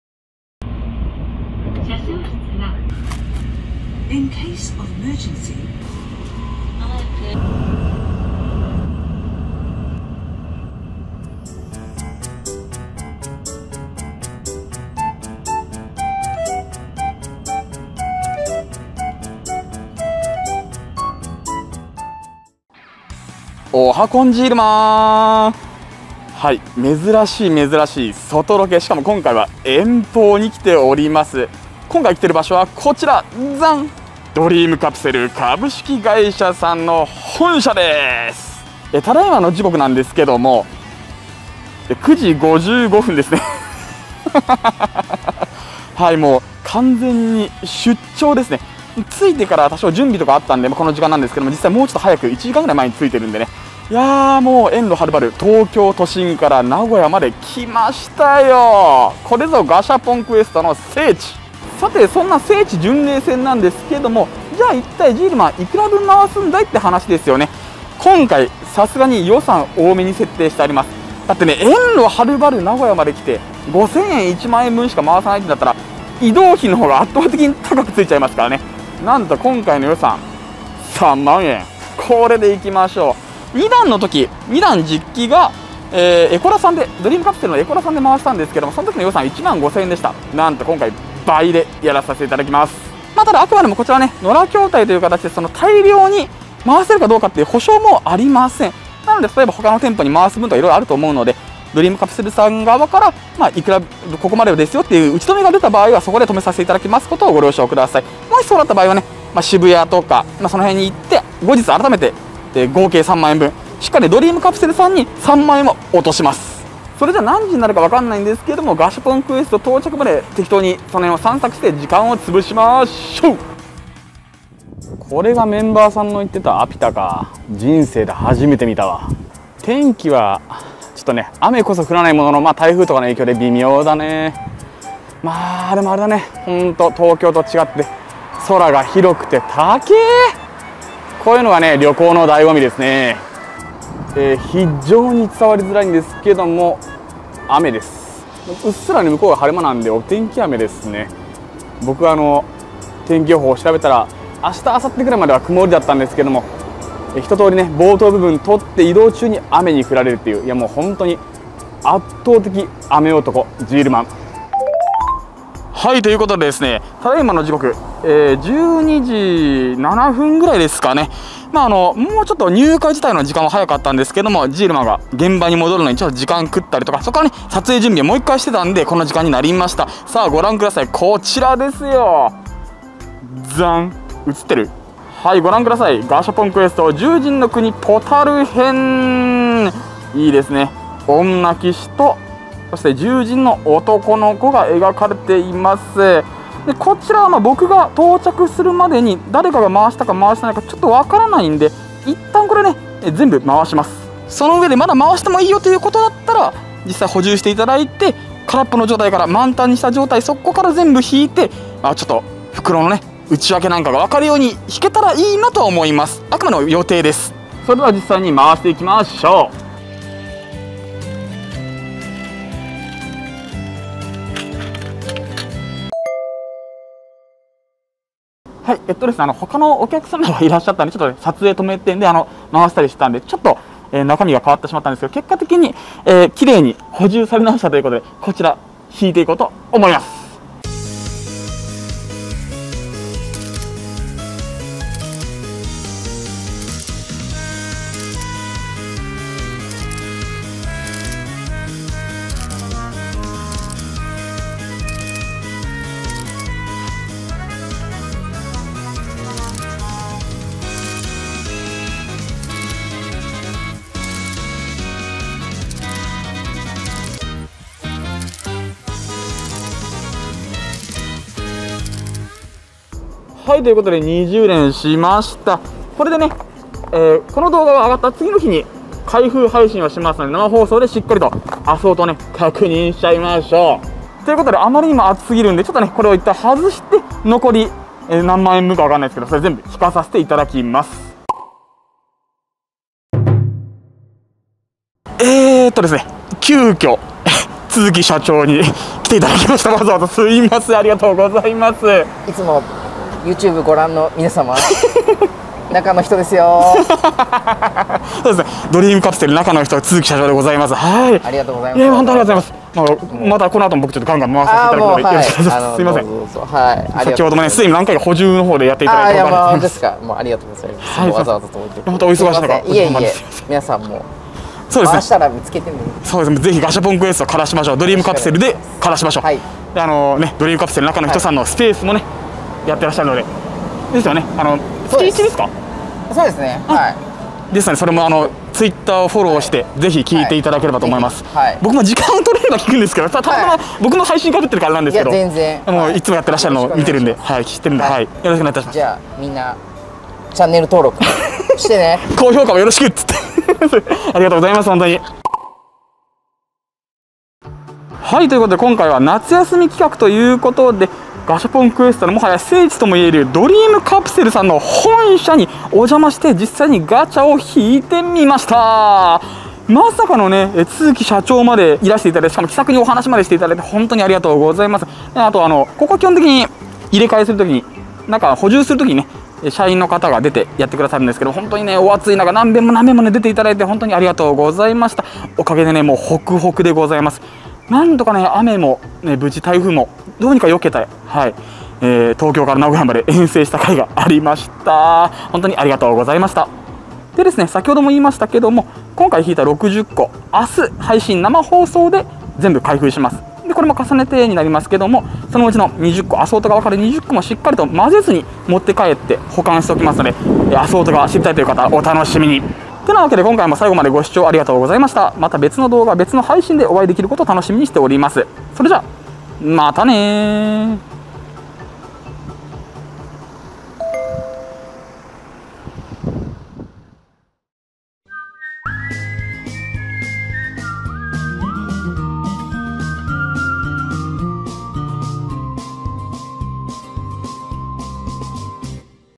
【ガシャポンクエスト3弾】ドリームカプセルさんの本社行ったら大変なことになった！！
動画の更新やお休み情報などがメインです。

おはこんじーるまん！
今回は世間が夏休みということで、企画として初の都外ロケに行ったぞ！
行き先はガシャポンクエストの聖地！名古屋にあるドリームカプセルさんの本社だ！！
事前確認の電話対応に当日の対応、更に社長との会話と全てが温かい会社でした！本当にありがとうございました！！
勿論ガシャポンクエスト実機版３弾も爆回し！６０回引いてほっくほくだぞ！！

使用機材：
SONY ZV-E10